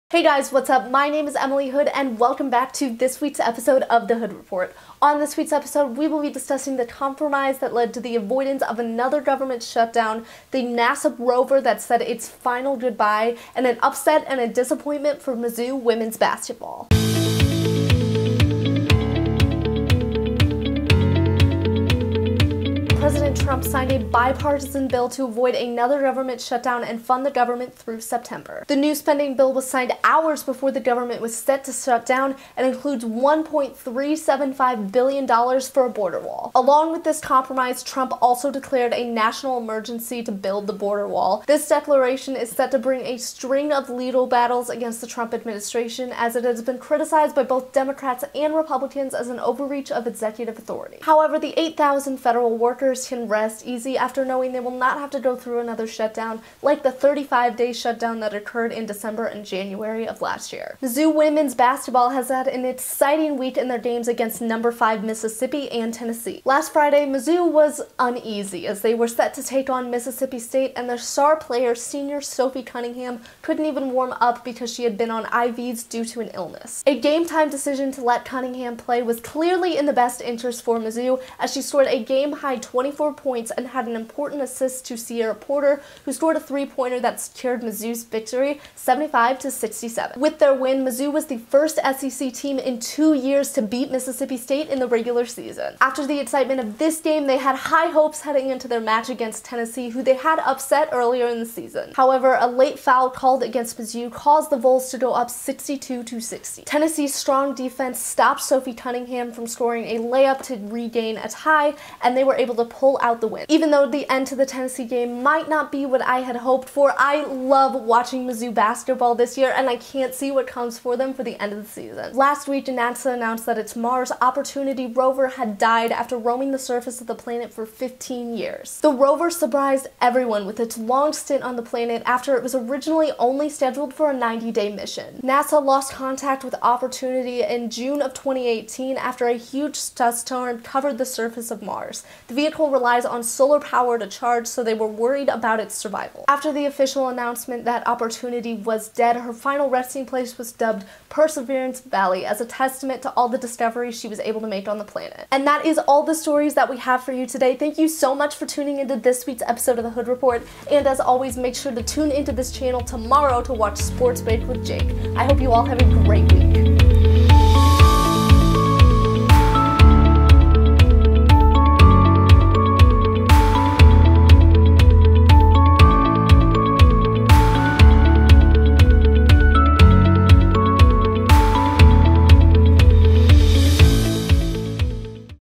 The Hood Report: Exciting Week for Mizzou Women's Basketball
On this week's episode of The Hood Report, anchor Emily Hood discusses the top events of the week, including the spending bill to avert another government shutdown and the national emergency declared by President Trump, the exciting week in Mizzou Women's Basketball, and the death of NASA's Opportunity Mars Rover.

ATTRIBUTIONS
Photos:
Opportunity Rover: NASA/JPL-Solar System Visualization Team [Public domain]

Icons:
Twitter, Instagram, and Facebook icons made by Freepik from flaticon.com
Website icon made by Alfredo Hernandez from flaticon.com